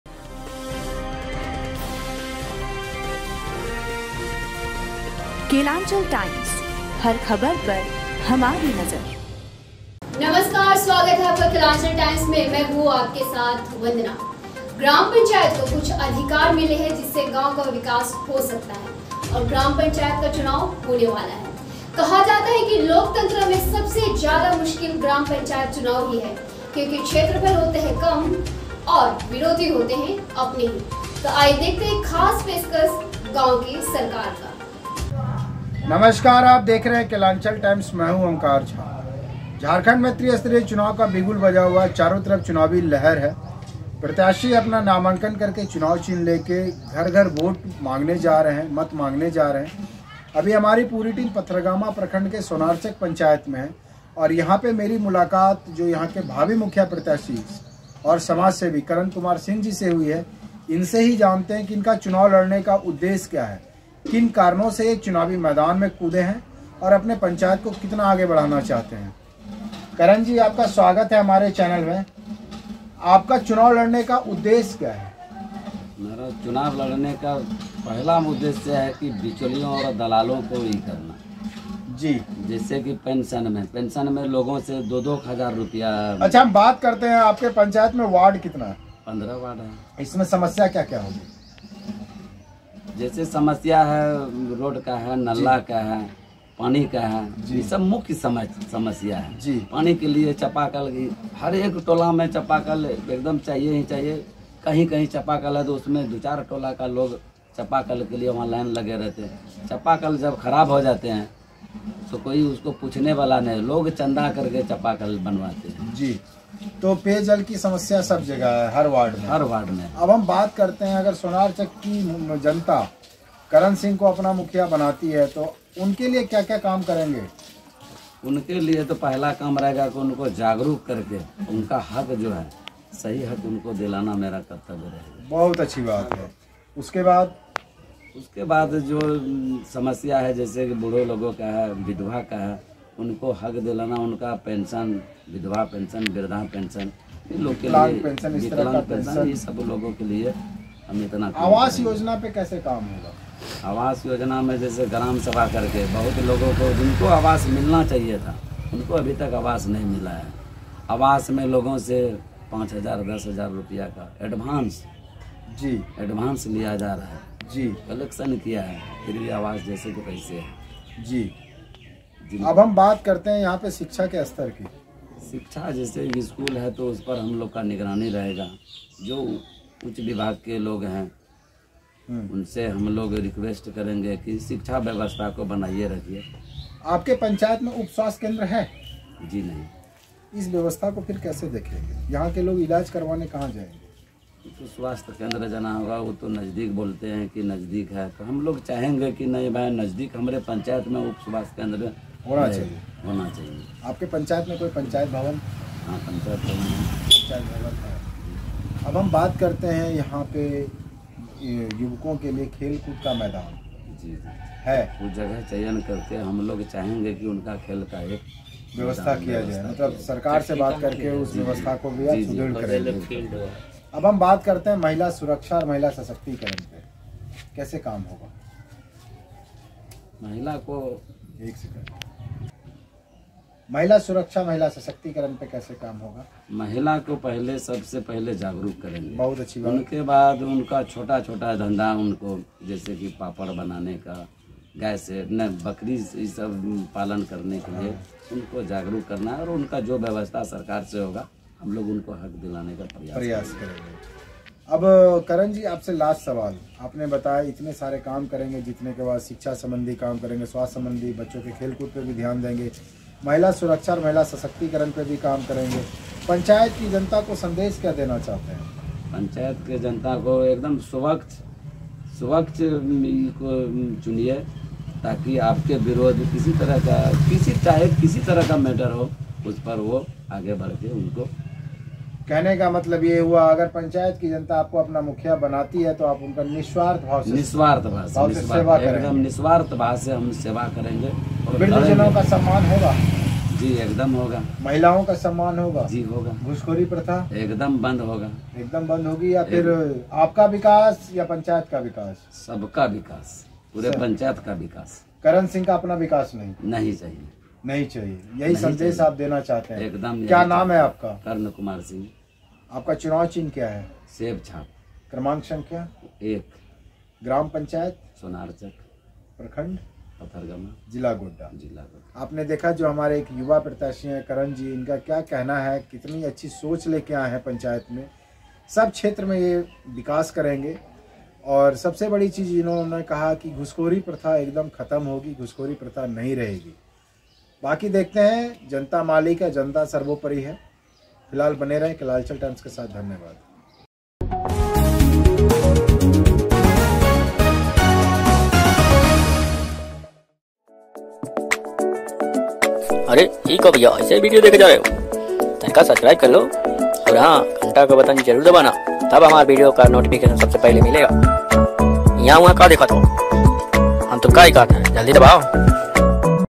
सोनारचक पंचायत निर्वाचन क्षेत्र संख्या -11 के भावी मुखिया प्रत्याशी कर्ण कुमार सिंह से खास बातचीत
पथरगामा प्रखंड के सोनारचक पंचायत निर्वाचन क्षेत्र संख्या -11 के भावी मुखिया प्रत्याशी कर्ण कुमार सिंह से संवादाता ओंकार झा की खास बातचीत